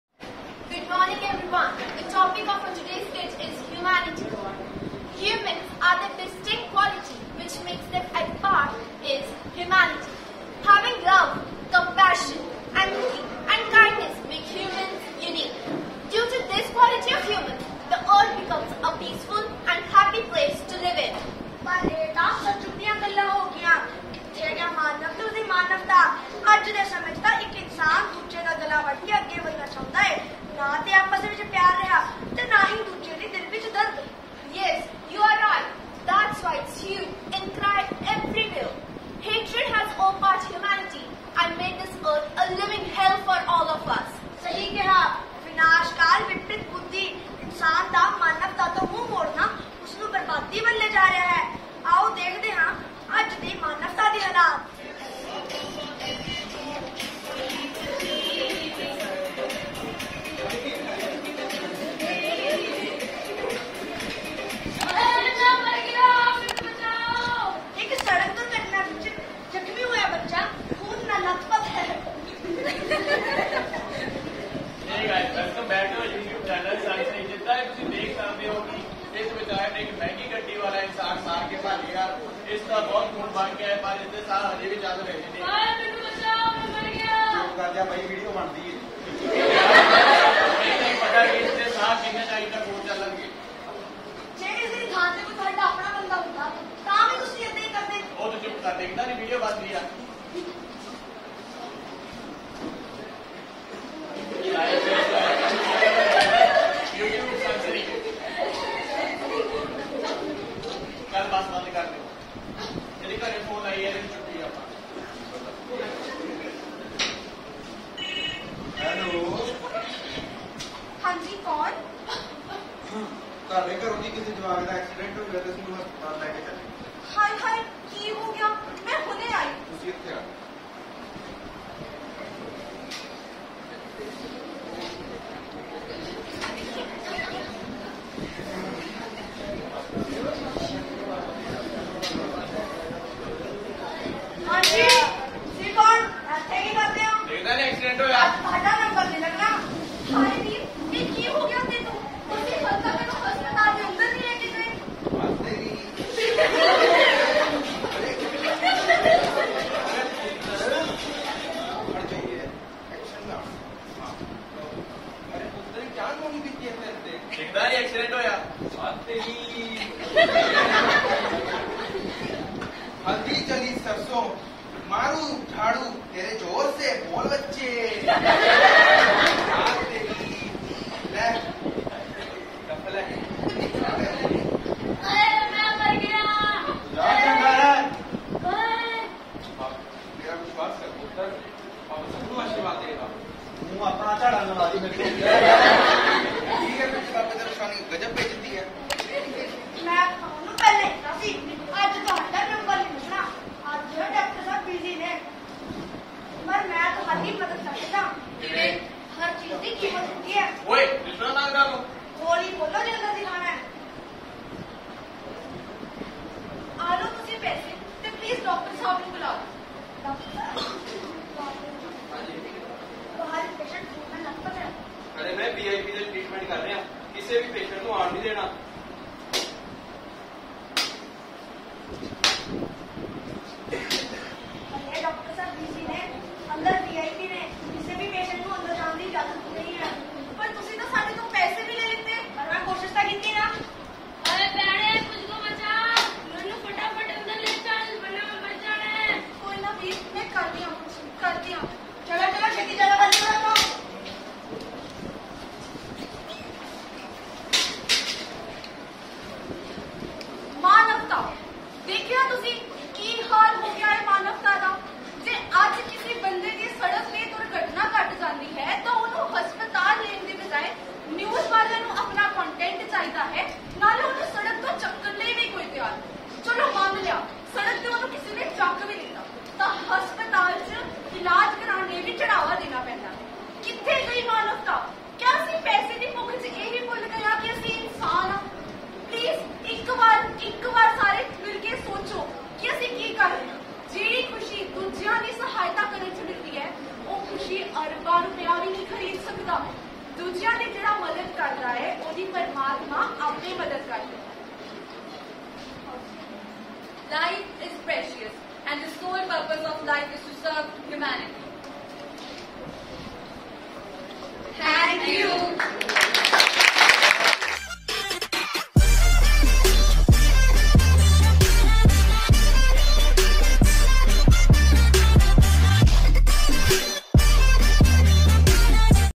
Skit on Theme 'Humanity' - A Performance by GNPS Students
Humanity is the human race, which includes everyone on Earth. It’s also a word for the qualities that make us human, such as the ability to love and have compassion, be creative, and not be a robot or alien.

Students of Guru Nanak Public School try to explain the real meaning of Humanity by their performance in Skit.